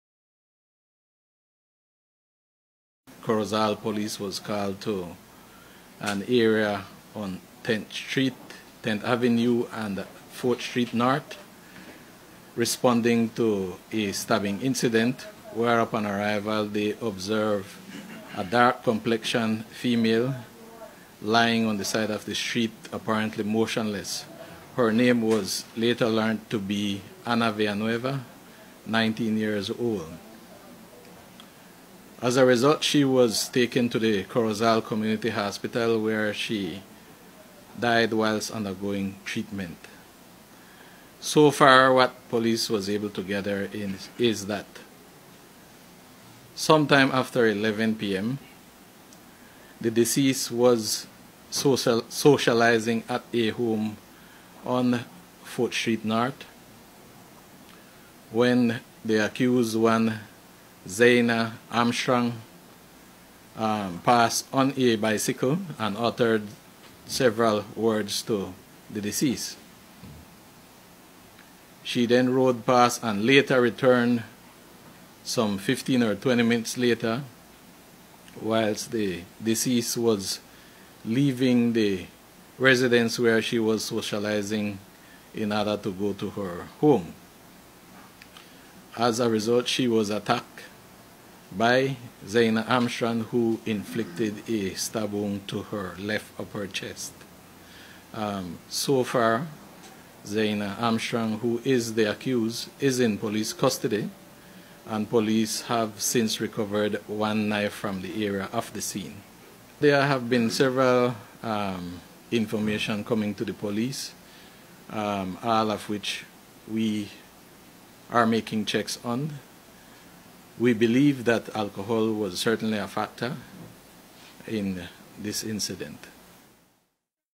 Teenage Girl Detained For Murder
A woman identified as Ana Villanueva, a resident of the Dominguez Layout in Corozal Town, was murdered last night, allegedly by another woman during a fight.